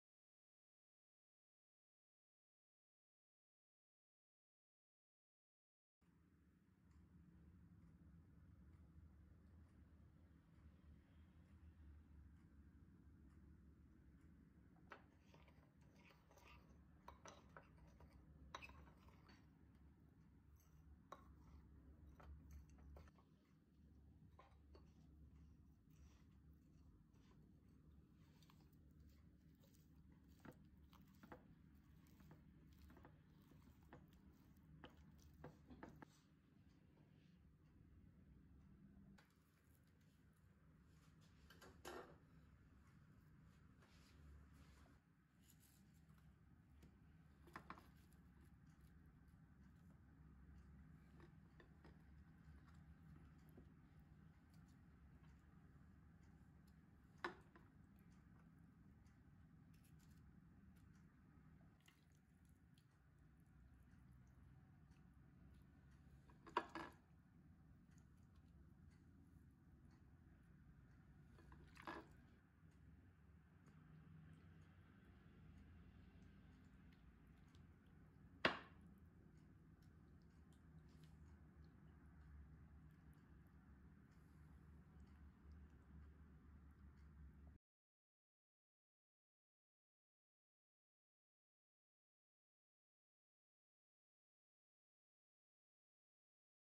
Most crunchy exquisite chicken so easy to make (Pui la cuptor crocant)
Chicken in the Oven with Vegetables

1,4 Kg Chicken
400 gm Mushroom
1 Onion
1 Capsicum
1 Tomato
Garlic
50 gm Butter
Coconut Milk
Salt, Pepper, Oregano, Paprika
In the Oven for 35 minutes at 200 degrees